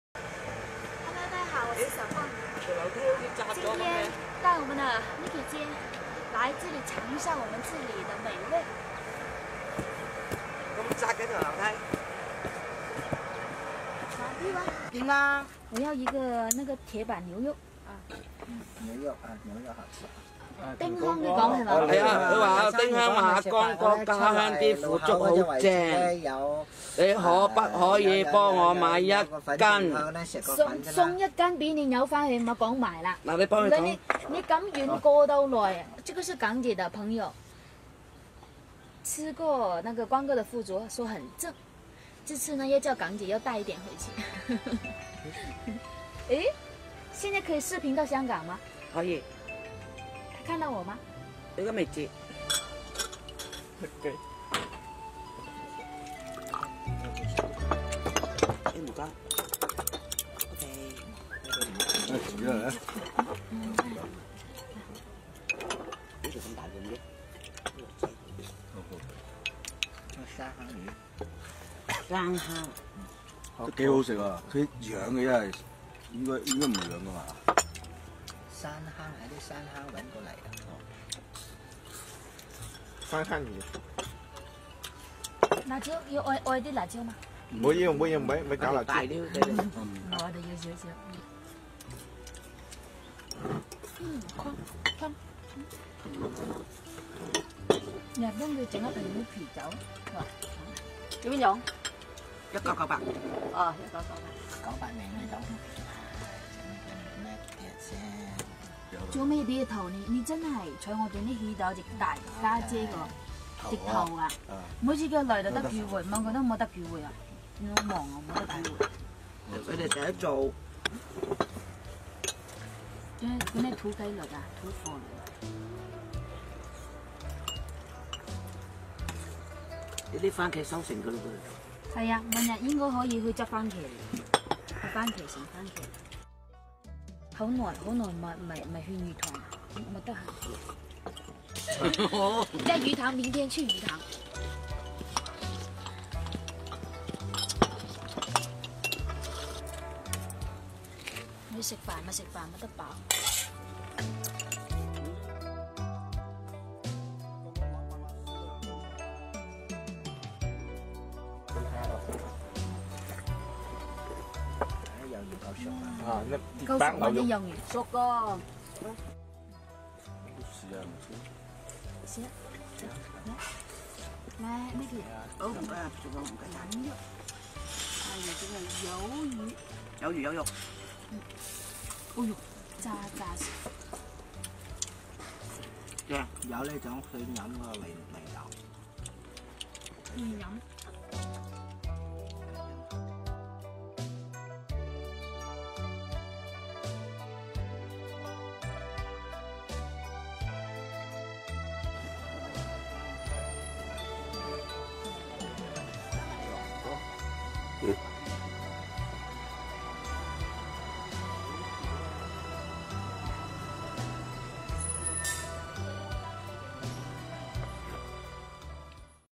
小鳳帶港姐來這裡招待，地方環境差點，但吃的土貨特別多【田野小鳳】
田野小鳳 是個專注 農村 農民農業的原創視頻